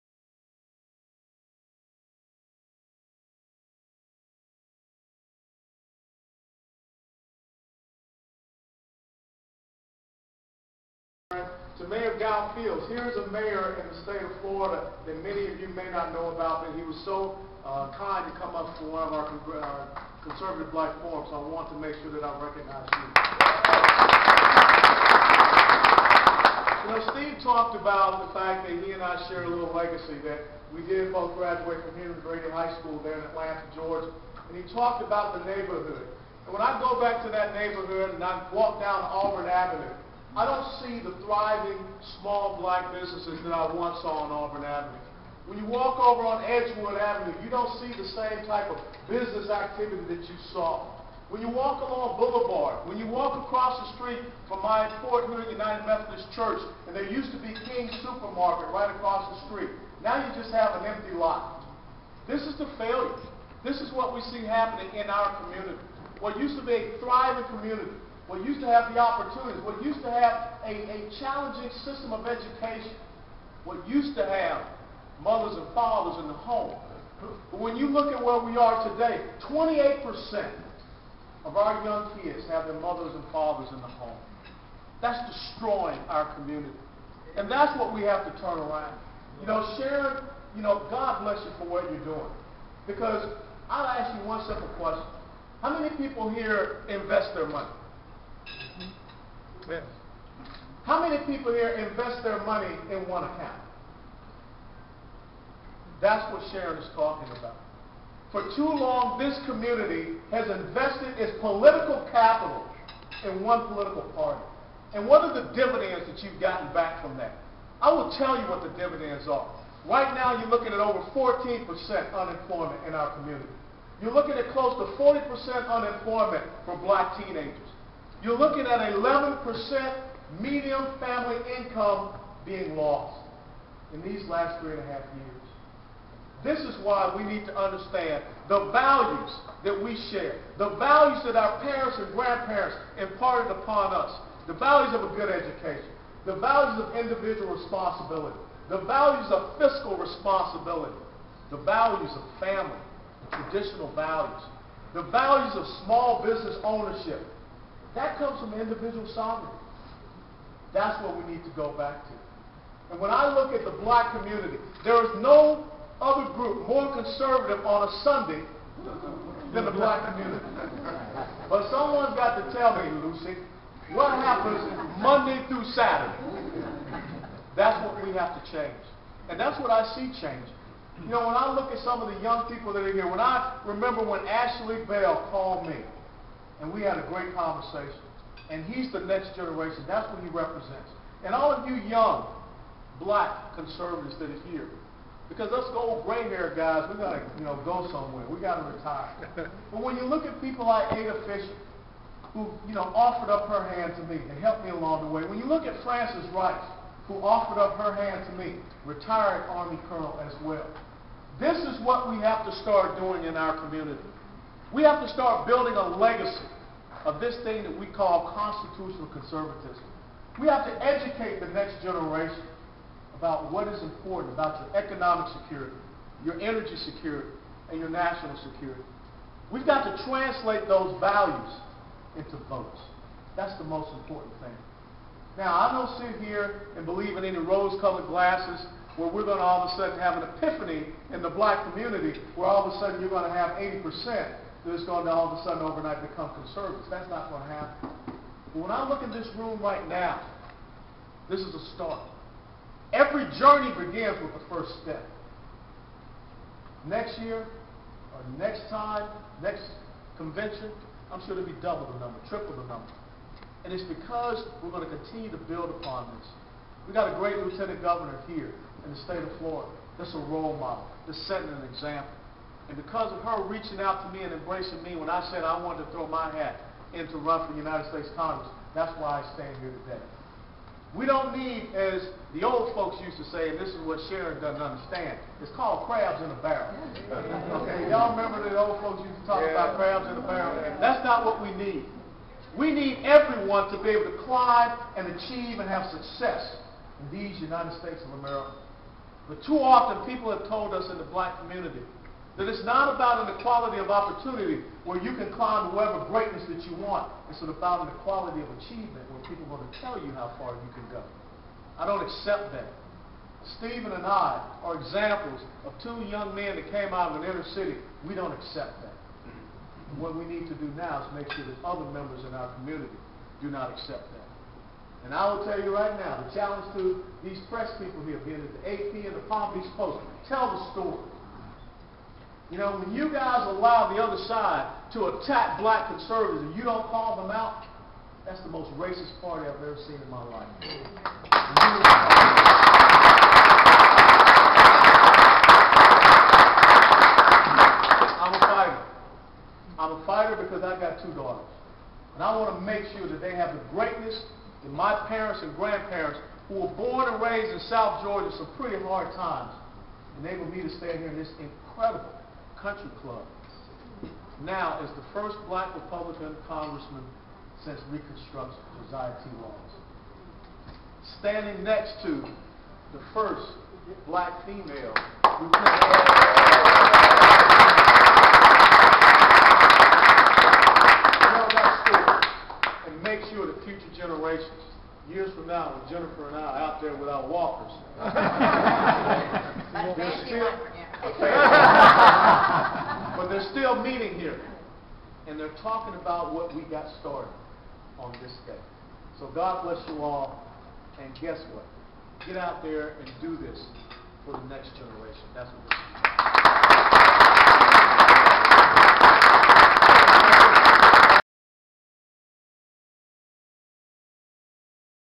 Rep. Allen West Speaks to Black Conservatives
Rep. Allen West shares a few thoughts with Black conservatives at the Republican National Committee's "Vision" Luncheon.